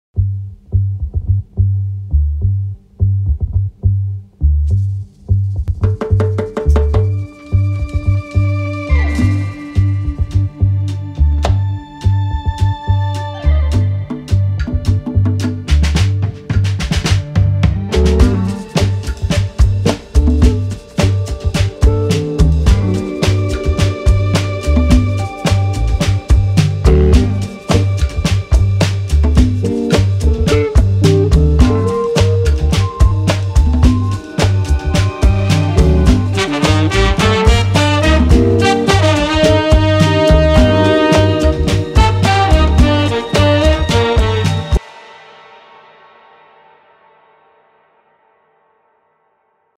TRAILER - THE HARD ROCK ROUNDABOUT APPRECIATION SOCIETY
Trailer for my latest concept film. The Hard Rock Roundabout Appreciation Society. Submitted for the ATVAS 2022. Fun